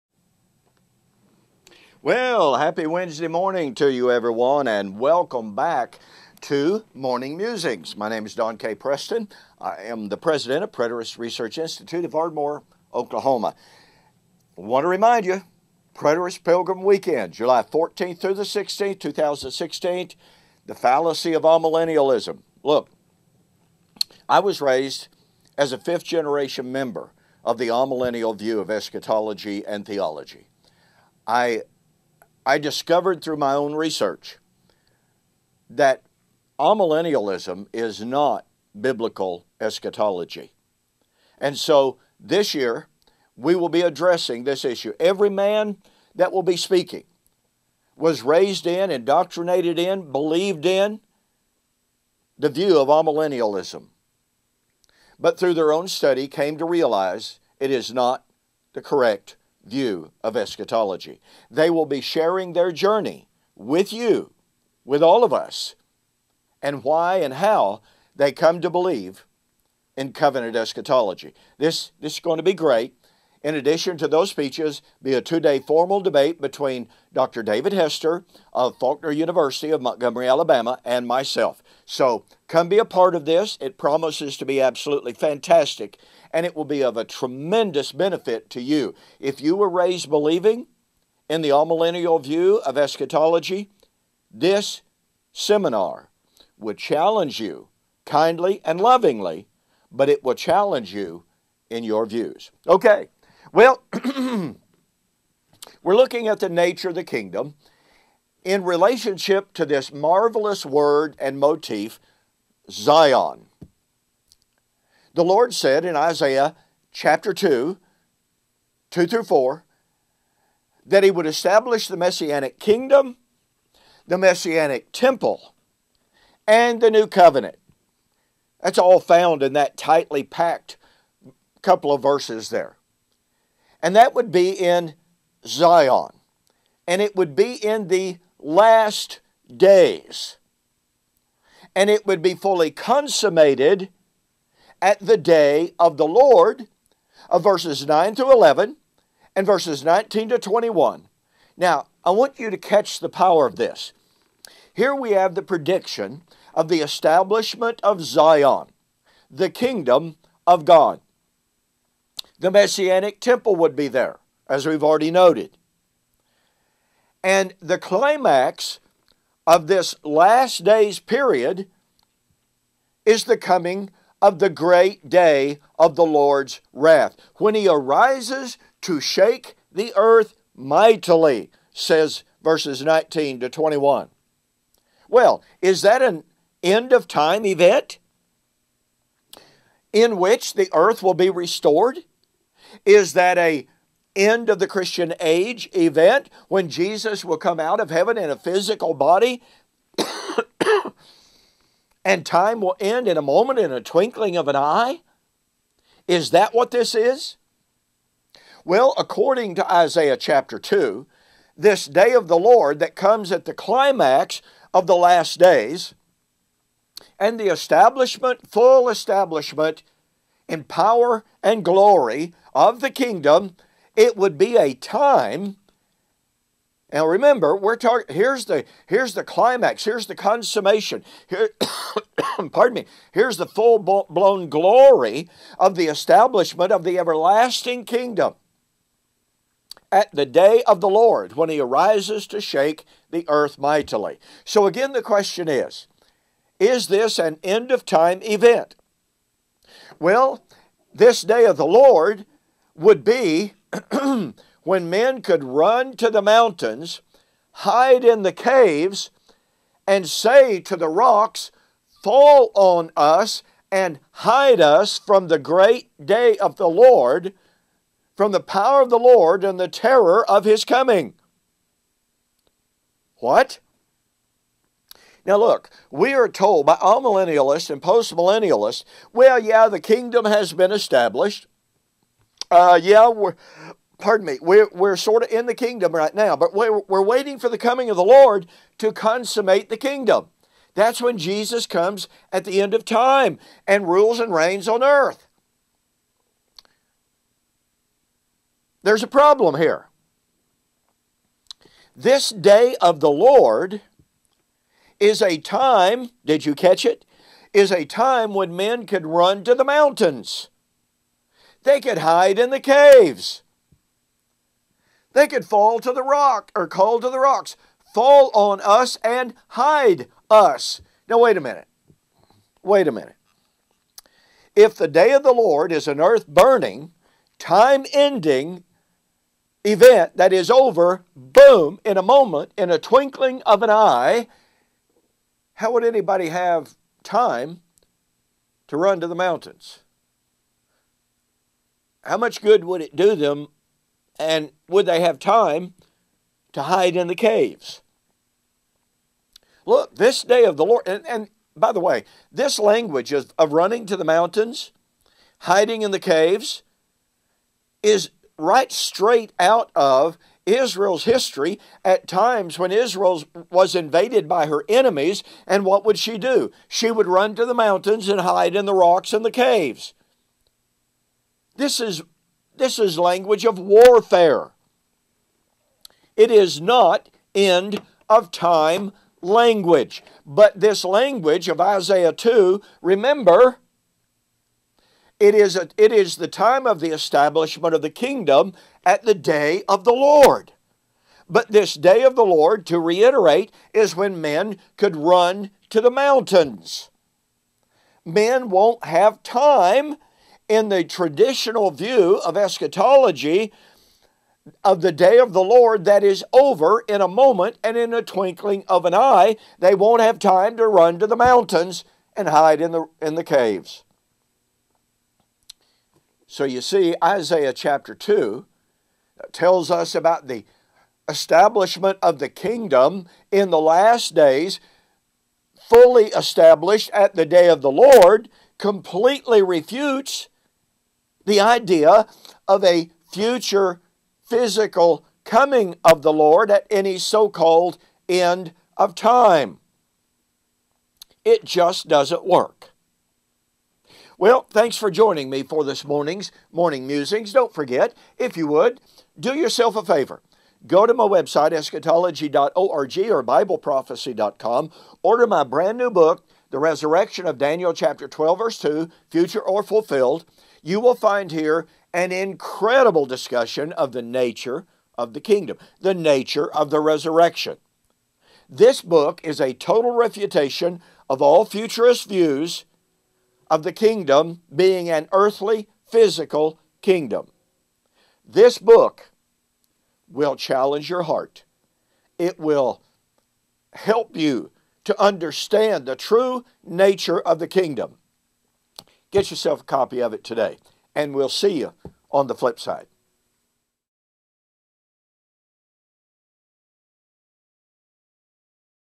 The Nature of the Kingdom #49 - Isa 2-4 - Run for the Hills!
Isaiah 2 foretold the Day of the Lord to climax the last days. What is the nature of that Day? is it an earth burning, time ending event in which Jesus comes physically? Not according to the text! Take a look, like it, and be sure to share it!